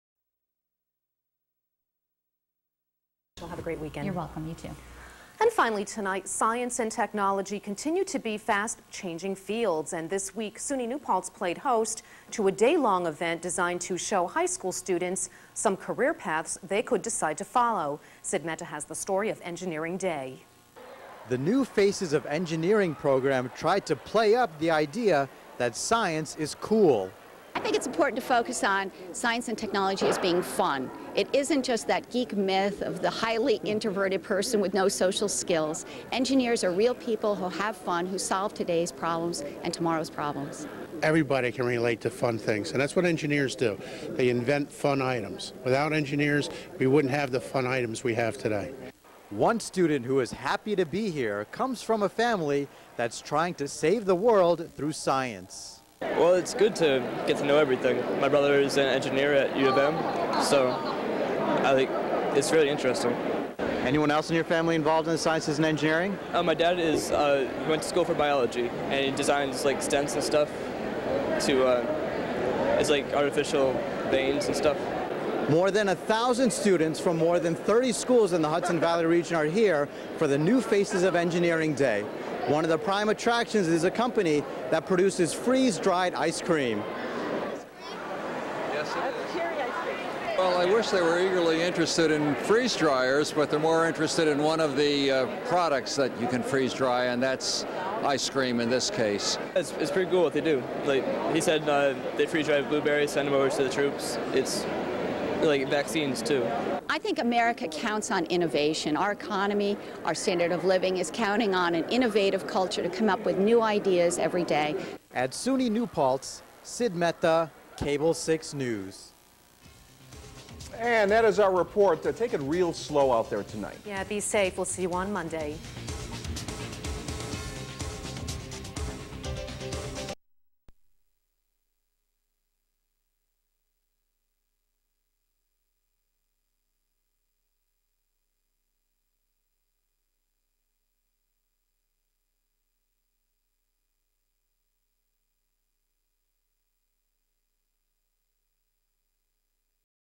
Engineering Day 2008
Engineering Day is designed for high school students to visit and explore possible career paths.